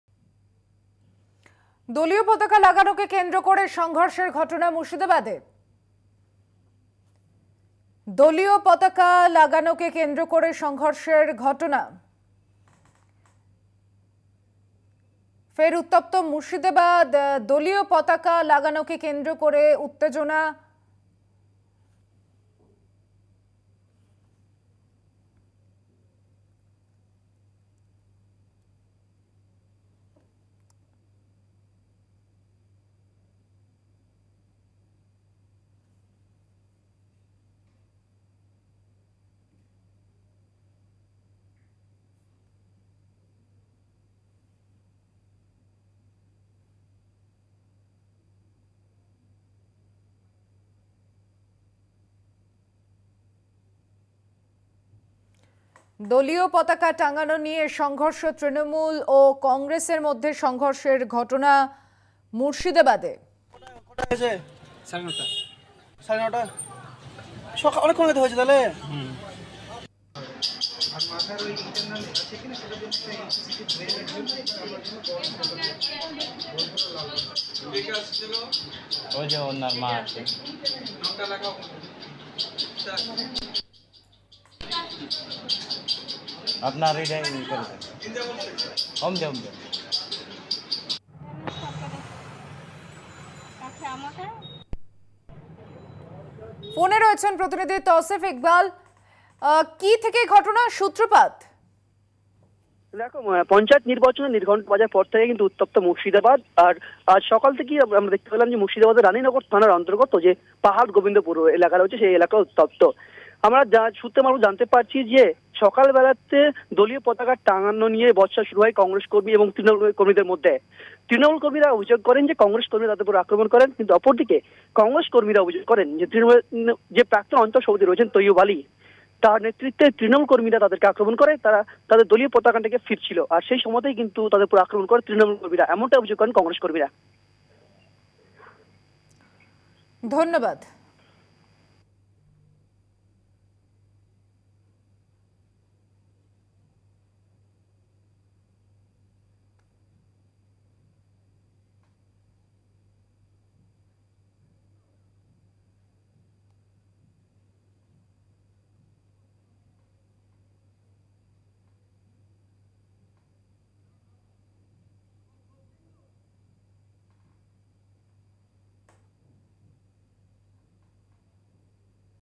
Murshidabad | TMC-Congress | দলীয় পতাকা লাগানোকে কেন্দ্র করে উত্তেজনা মুর্শিদাবাদে
website link for this copy :
@Kolkata Tv